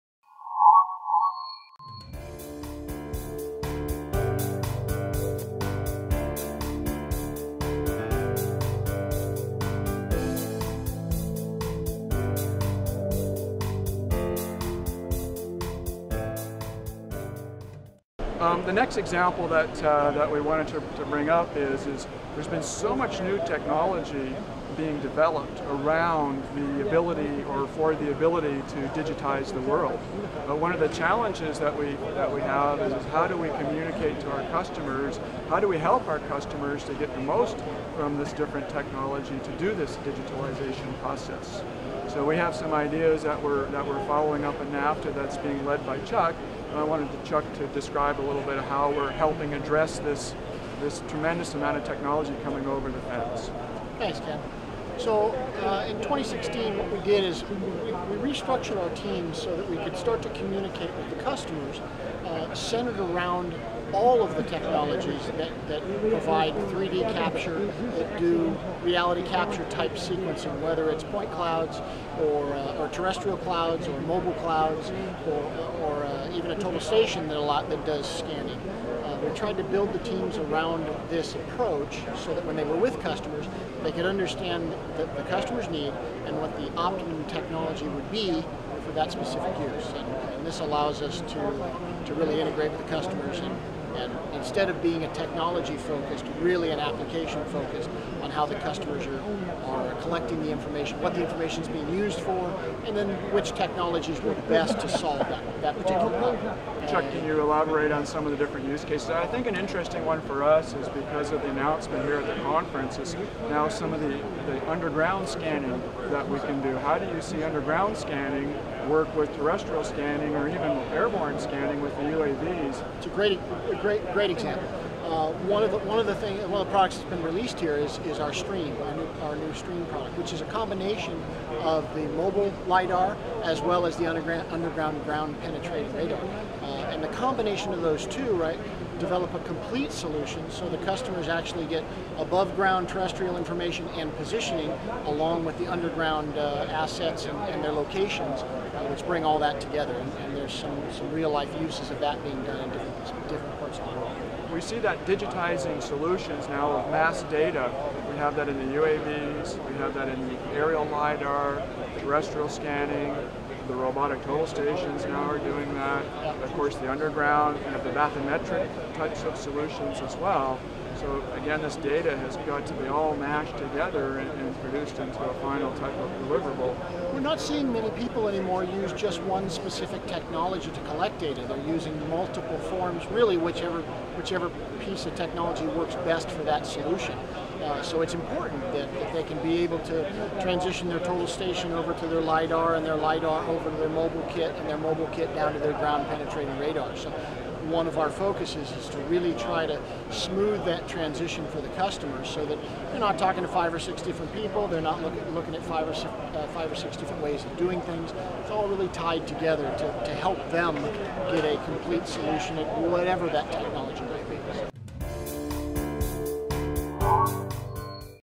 Ken Mooyman at HxGN LIVE 2016 - HDS and digitization (Part 4 of 4)
Digitization from multiple platforms. In this the forth of a four part interview with Hexagon (Leica) Geosystems Ken Mooyman he speaks with Chuck Coiner, HDS VP and division manager at Hexagon Geosystems  “We’re not seeing many people anymore using just one specific technology; they are using multiple forms” said Chuck. Citing the UAS, airborne, GPR, mobile systems, backpack, total stations and combinations thereof that can feed a complete 3D models; Chuck explained that his division reformed teams in 2016 to be able to better support these multi-tech approaches.